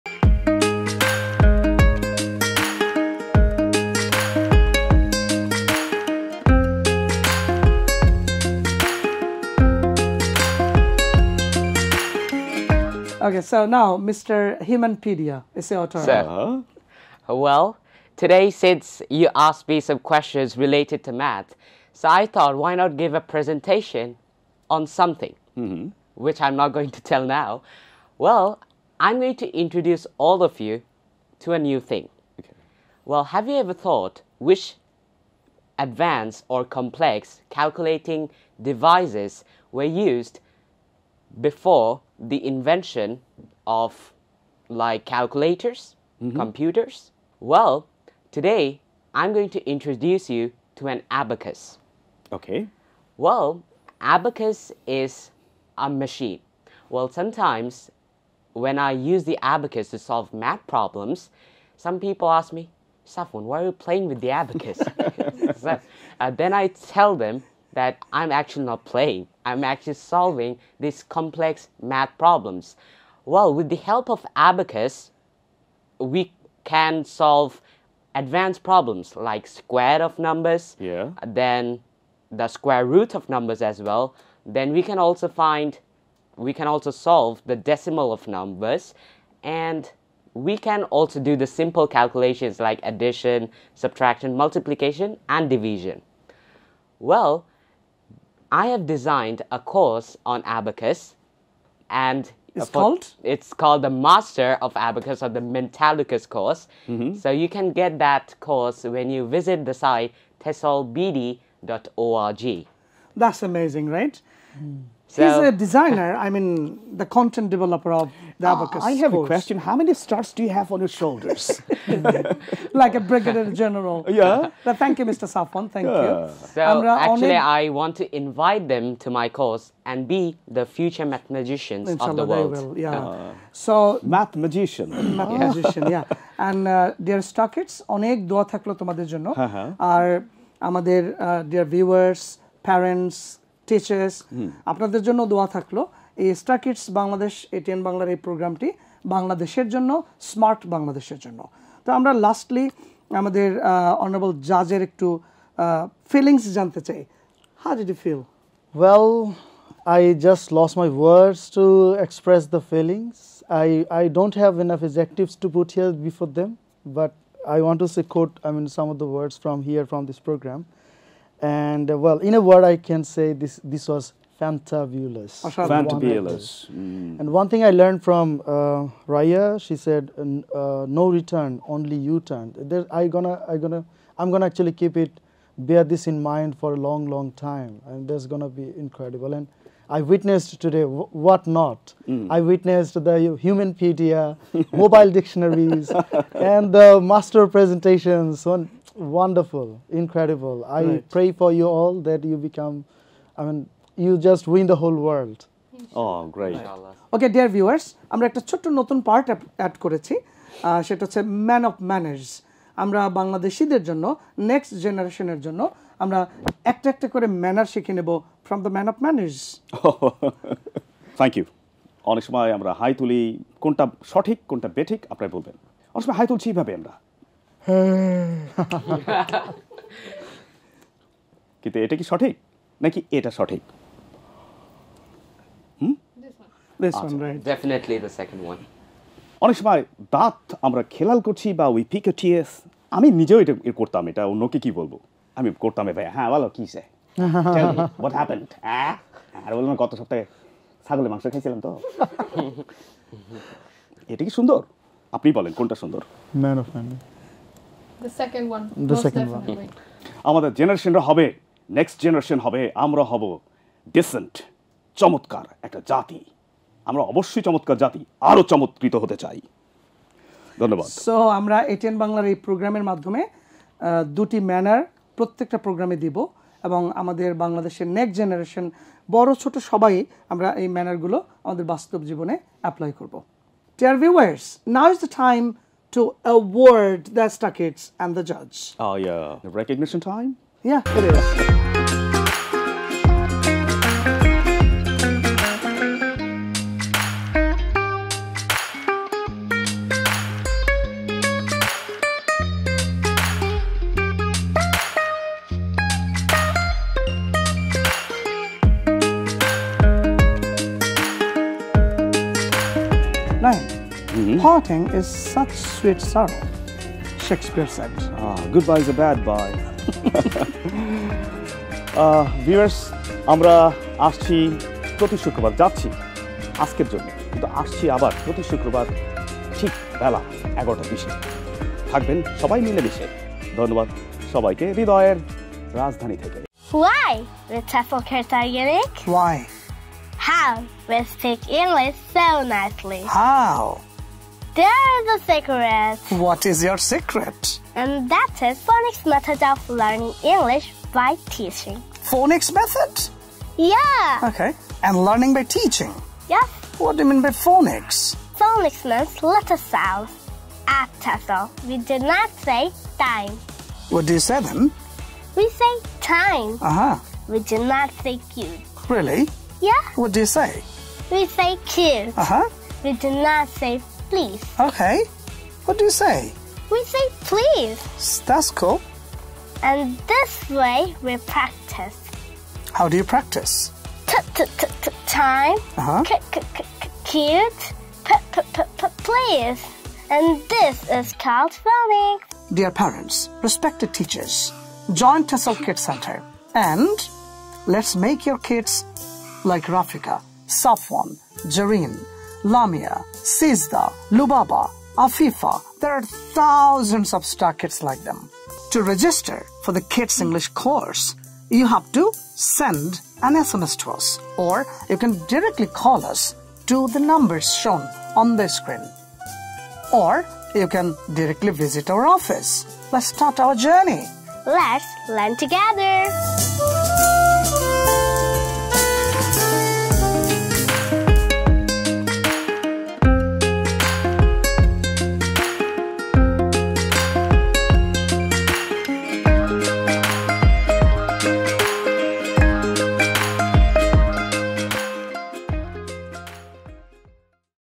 STAR KIDS BANGLADESH | Abacus | TESOL BANGLADESH
Description:
Introducing STAR KIDS BANGLADESH: Abacus, a fun and engaging program designed to enhance your child's mental math abilities. Join us at TESOL BANGLADESH as we help your child master the ancient art of abacus, developing lightning-fast calculations, improving their concentration, and boosting their self-confidence.